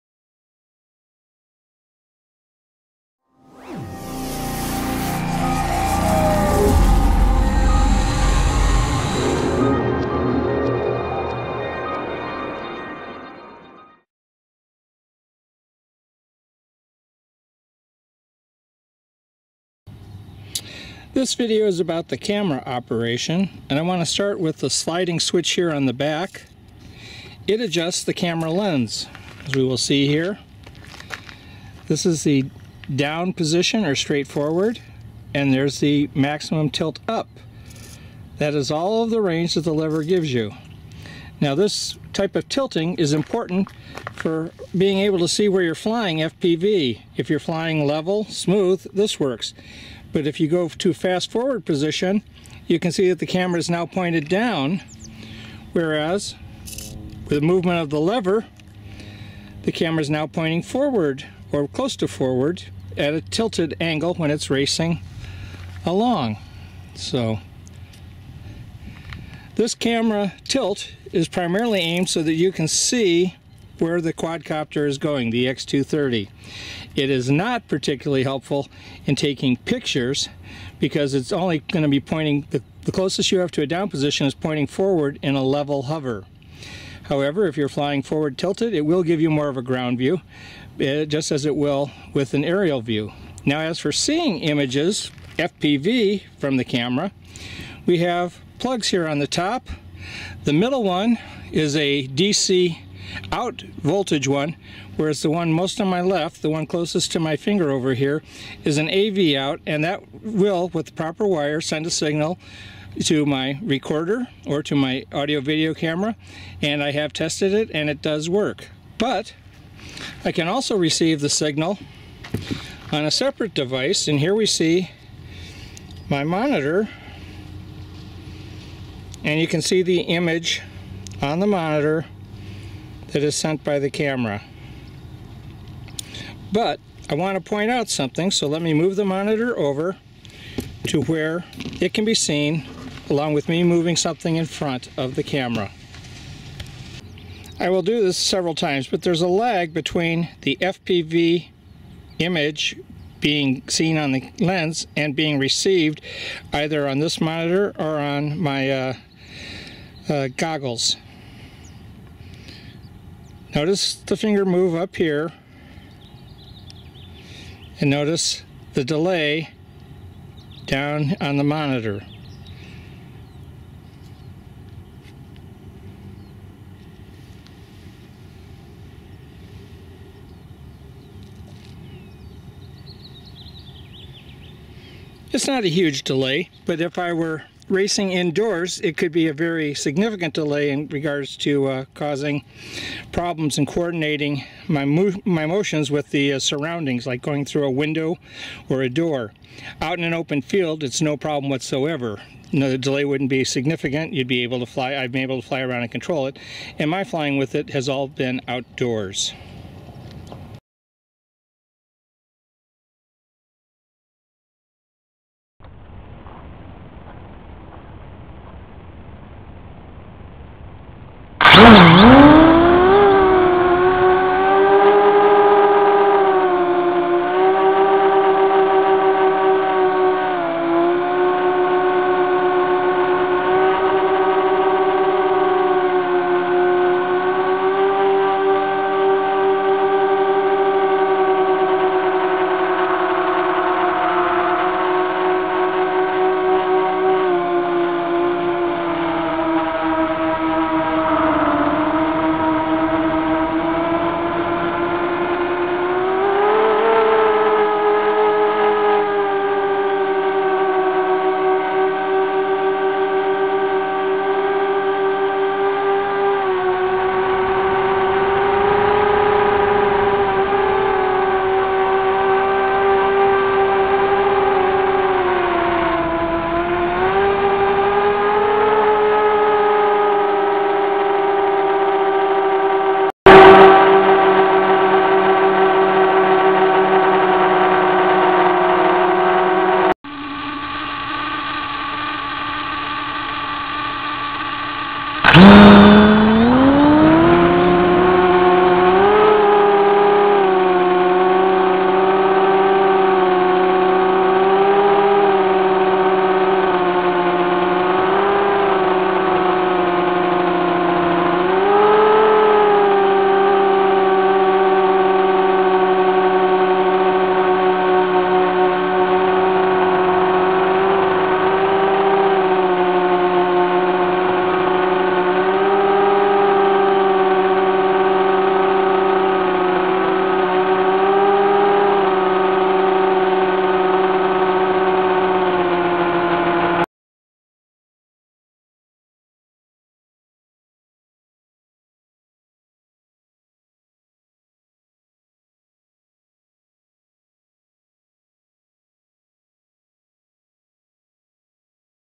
DYS X230 Quadcopter Camera Demonstration
A video showing the adjustable camera lens, FPV coverage, still pictures and the quality of the video. This is part of a review that will be in RC Groups in late February 2016. See the discussion there that goes with this video. Also a flight video.